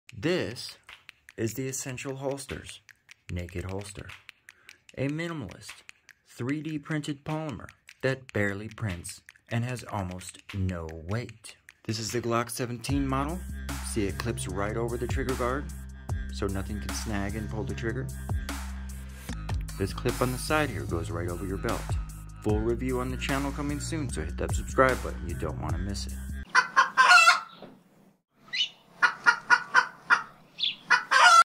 Naked holster
First look at the Essential holsters Naked holster
Full review coming soon